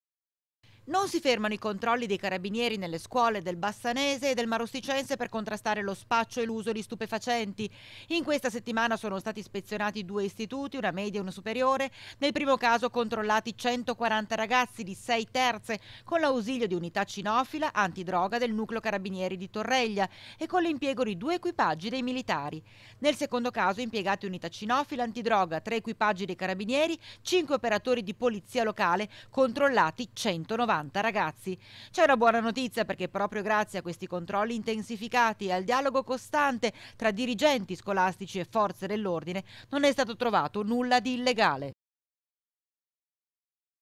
TG BASSANO (26/01/2019) - BLITZ ANTIDROGA IN DUE SCUOLE, CONTROLLATI PIU' DI 300 STUDENTI
TG BASSANO (sabato 26 gennaio 2019) - Non si fermano i controlli dei carabinieri nelle scuole del bassanese e del marosticense, per contrastare lo spaccio e l'uso di stupefacenti. In questa settimana sono stati ispezionati due istituti, una media e una superiore. Nel primo caso controllati 140 ragazzi di 6 terze, con l’ausilio di unità cinofila antidroga del Nucleo Carabinieri di Torreglia e con l’impiego di due equipaggi dei militari. Nel secondo caso impiegate unità cinofile antidroga, 3 equipaggi dei carabinieri e 5 operatori di polizia locale, controllati 190 ragazzi.
C'è una buona notizia perchè proprio grazie a questi controlli intensificati e al dialogo constante tra dirigenti scolastici e forze dell'ordine, non è stato trovato nulla di illegale.